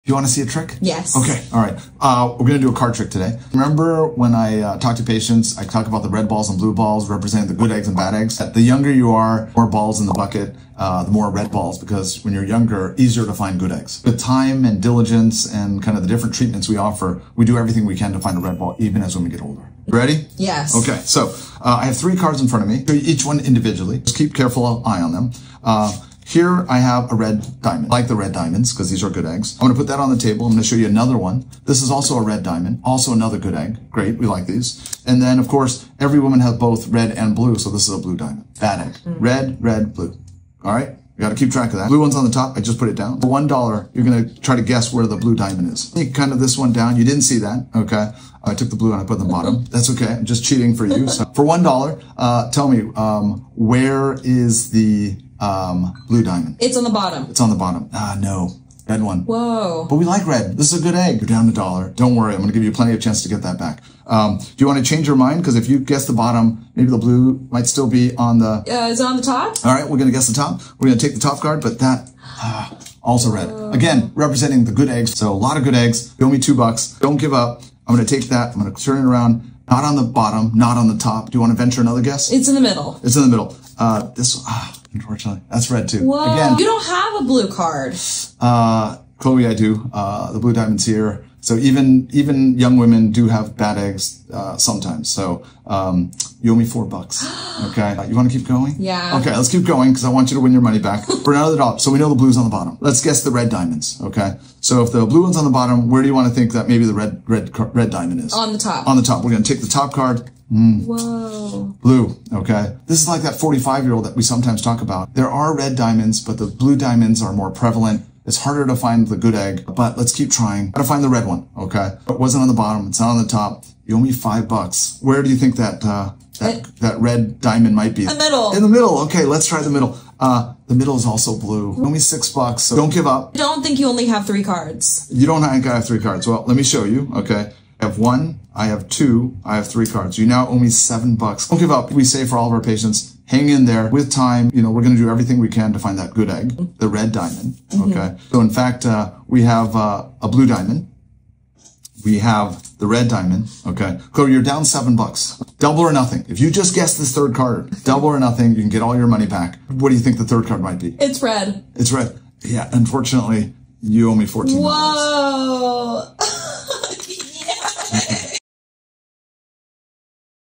🎃 Fertility Isn’t Magic — It’s Science! Dr. Andy Huang on Egg Quality Explained
Fertility may not be magic, but with experts like Dr. Andy Huang, it can certainly feel that way. 🪄

Wishing you a safe and spirited Halloween from all of us at Reproductive Partners! 🎃